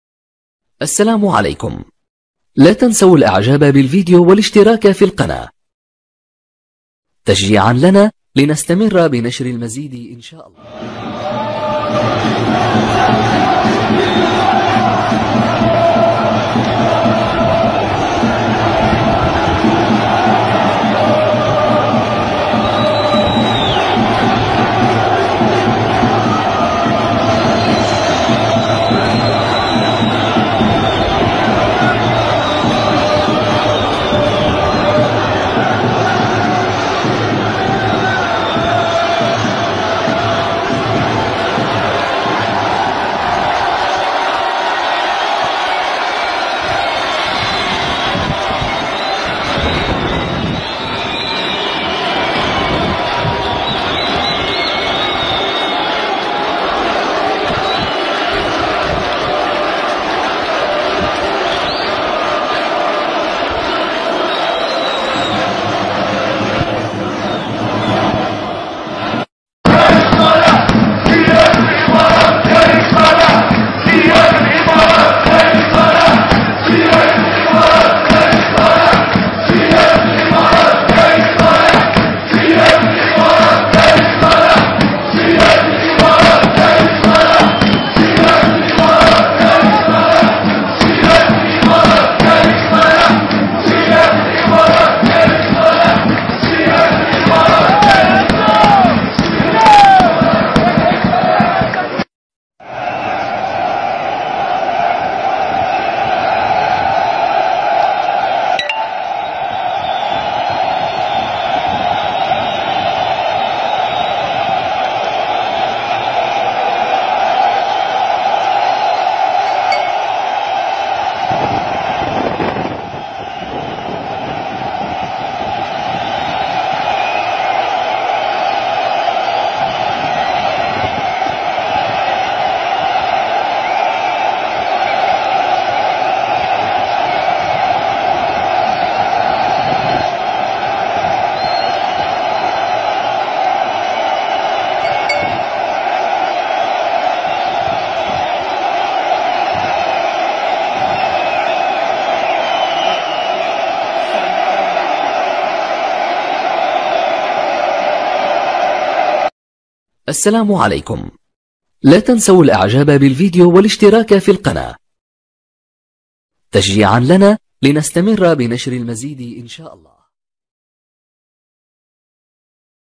مسيرة اليوم 14 جوان الجمعة 17 البريد المركزي الآن شاهد كيف تم اختراق حاجز الشرطة بقوة
مسيرة اليوم 14 جوان من البريد المركزي الجزائر العاصمة رغم الصيام والجو الحار لم يبالي الشعب بذلك .